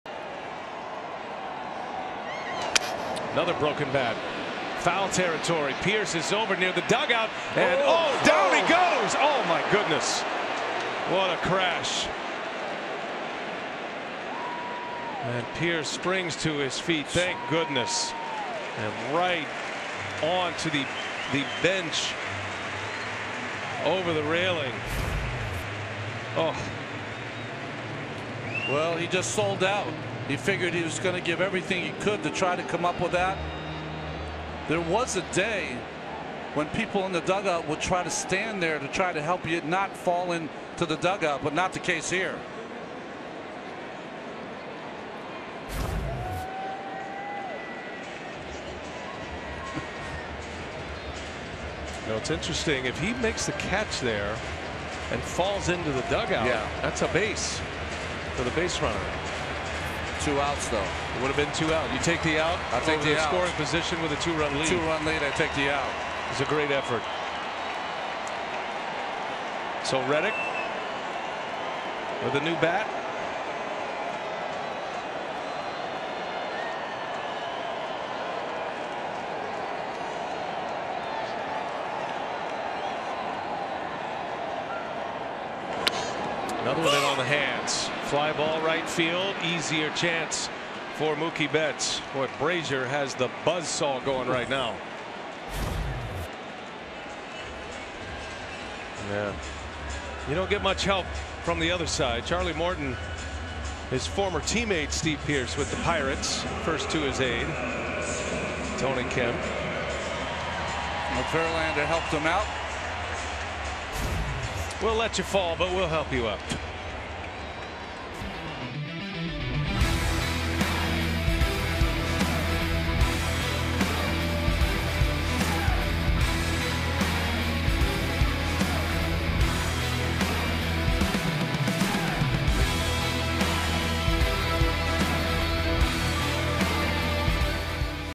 Steve Pearce flips over railing in attempt to make a catch
Red Sox first baseman Steve Pearce attempts to make a catch near the dugout as he flips over the railing